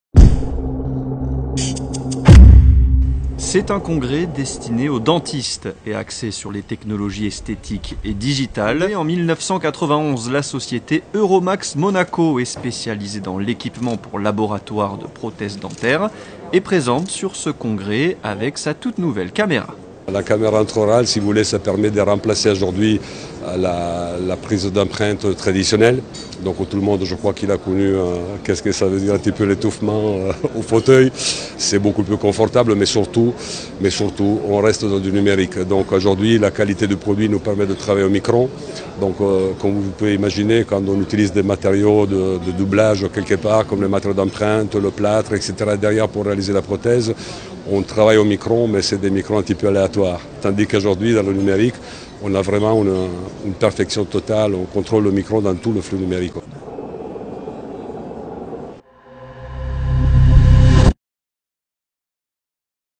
imagina dental
Imagina Dental : Interview de Massimo Gai - Président de Euromax Monaco sur Monaco Info.